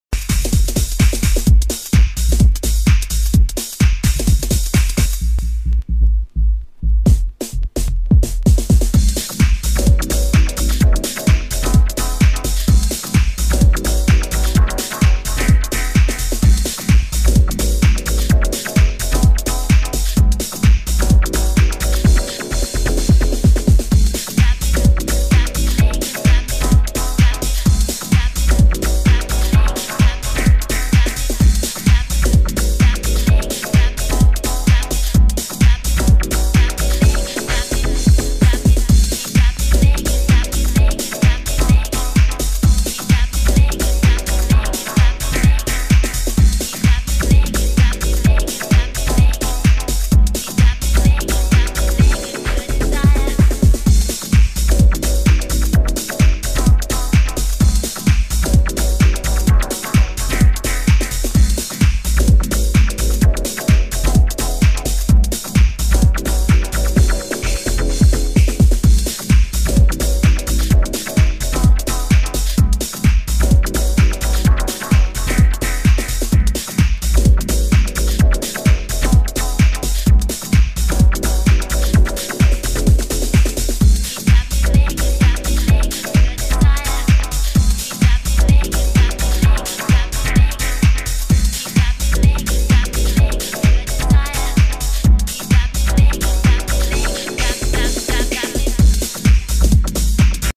Please ID this 90s underground house tune.
From GarageVybez98's vol 32 mix.